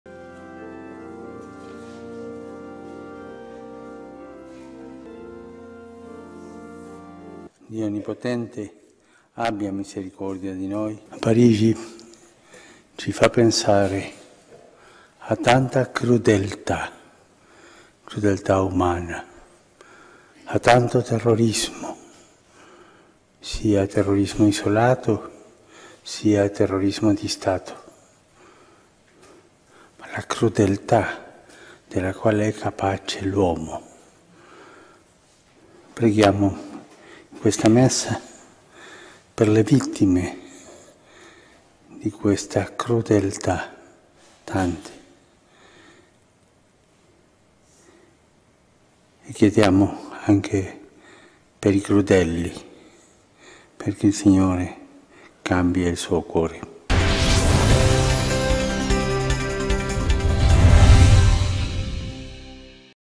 El Papa denunció la "crueldad humana" y envió su pésame por el atentado al semanario francés
El papa Francisco denunció hoy la "crueldad humana" en el atentado contra la revista francesa Charlie Hebdo y le envió un telegrama al arzobispo de París en el que da el pésame a las familias de las víctimas.

"El atentado de ayer en París nos hace pensar en la mucha crueldad humana. En tanto terrorismo, tanto el aislado, como el de Estado", afirmó el pontífice durante la misa matutina que oficia en la Casa Santa Marta, informó Radio Vaticano.

"¡De cuánta crueldad es capaz el hombre!", exclamó Francisco, para después invitar a los presentes a rezar por las 12 víctimas y los heridos del ataque de ayer en la redacción de Charlie Hebdo.Además, la oficina de prensa del papa comunicó que el pontífice expresó en un telegrama el pésame a las familias de las víctimas del atentado y se unió a su dolor y "a la tristeza de todos los franceses".

La misiva fue enviada por parte del secretario de Estado, Pietro Parolin, al arzobispo de París, el cardenal André Vingt-Trois, y publicado por la oficina de prensa."Su Santidad Francisco se une en oración al dolor de las familias de las víctimas y a la tristeza de todos los franceses", expresa el telegrama que reprodujo la agencia EFE.

Jorge Bergoglio también reitera "su condena de la violencia que genera tanto sufrimiento" y pide a Dios "que otorgue el don de la paz".

El Papa, continúa el mensaje, "confía las víctimas a Dios, lleno de misericordia, que les acogerá en su luz", y también expresa "el más sentido pésame a los heridos y para sus familias" y pide "al Señor que les de alivio y consuelo en su prueba".